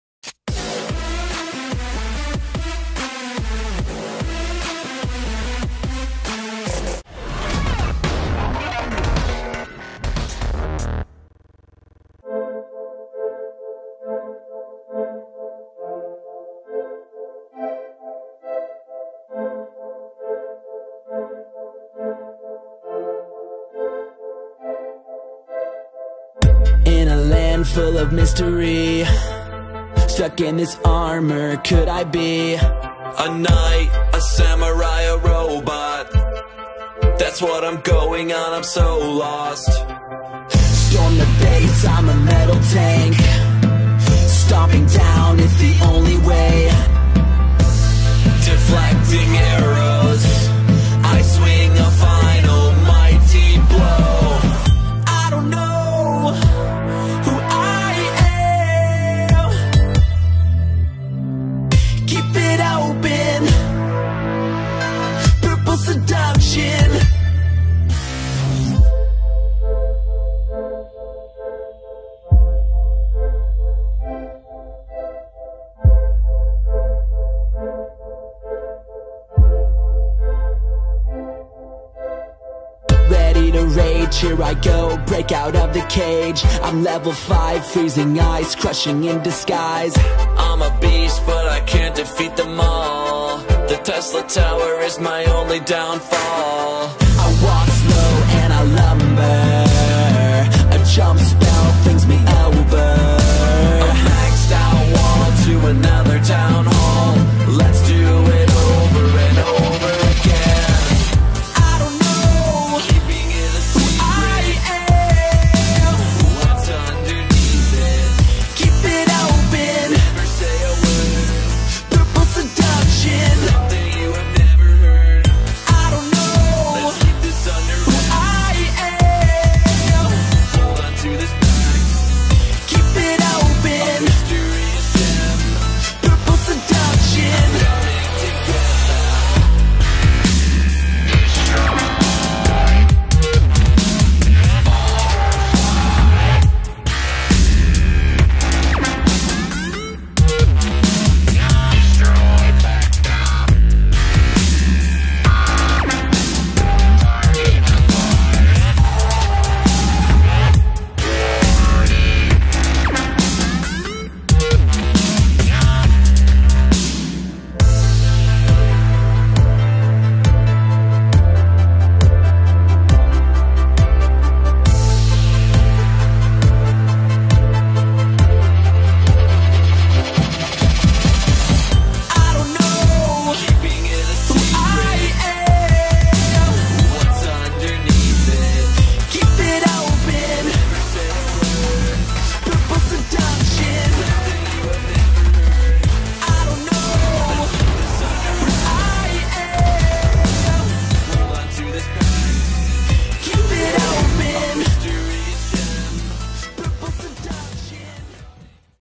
GOWIPE + HOGG 3 STAR STRATEGY ON TH8!!!!!!
THIS IS A LIVE VIDEO ATTACK IN WAR! WE WILL UPLOADS NWE VIDEOS FOR ALL OF U.